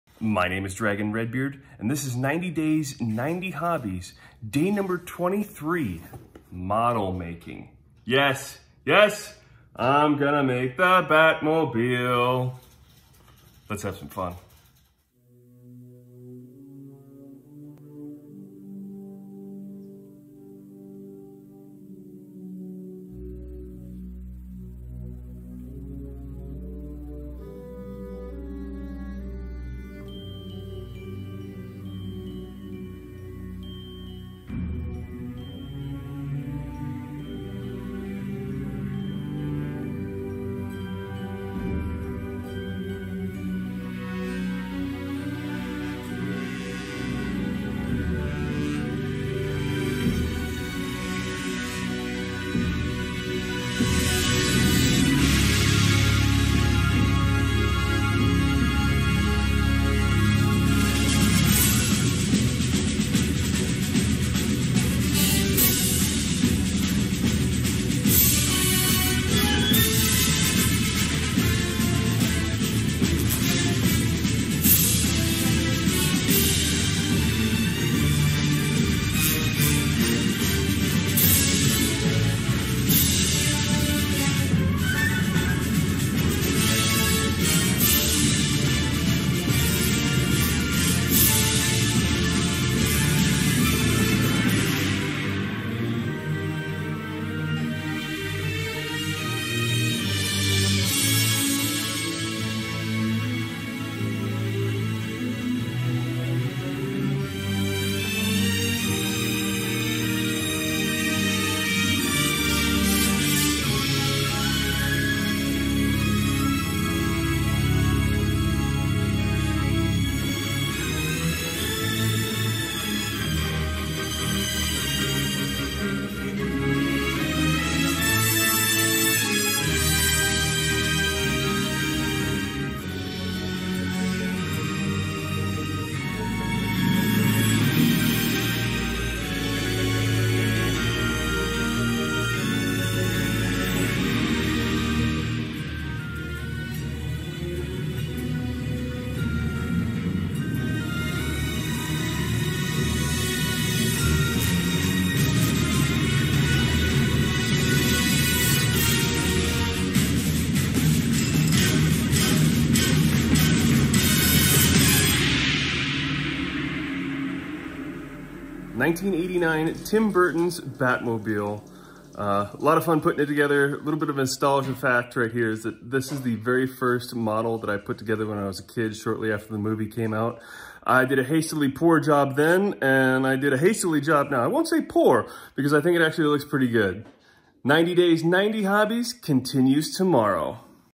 90 Days, 90 Hobbies: Model Making
This honestly brought back A LOT of memories. I made this very same model when I was kid shortly after the 1989 Batman Movie came out. To all of us 80's and 90's kids, this will always be our Batman (along with the animated series). This was a lot of fun, keep up the good suggestions of hobbies for me to try!